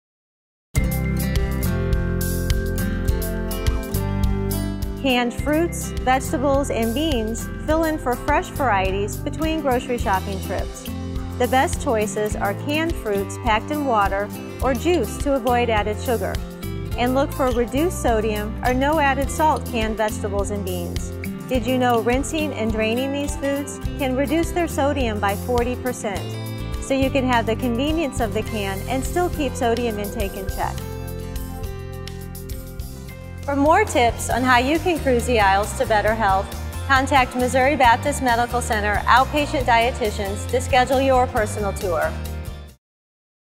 Shop Talk in the Canned Fruits and Vegetables Aisle
Canned fruits and vegetables fill in for fresh produce between grocery store visits.